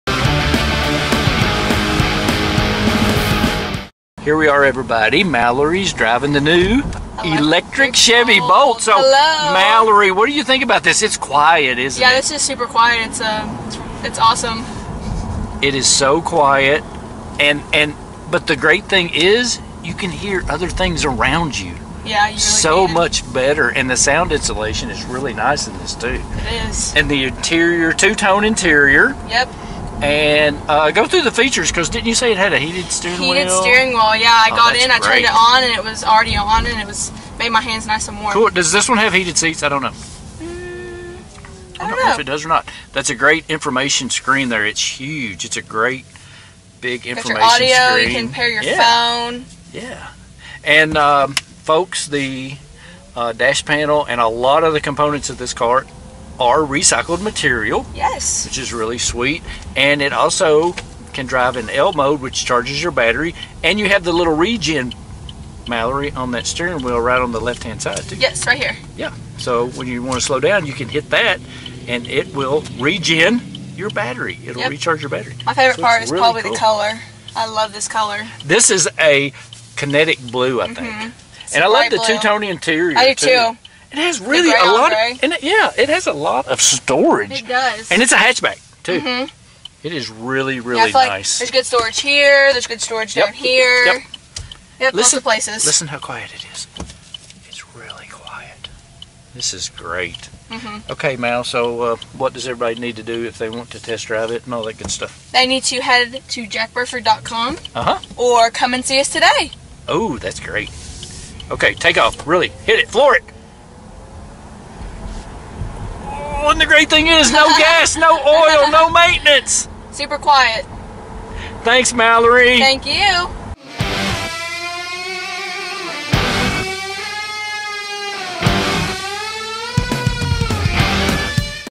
Mallory Test Drives a New Chevy Bolt EV
Mallory Test Drives a New Chevy Bolt EV - All New Chevy Bolt EVs - FREE TEST DRIVE in these ALL ELECTRIC VEHICLES! Never pay for gas again!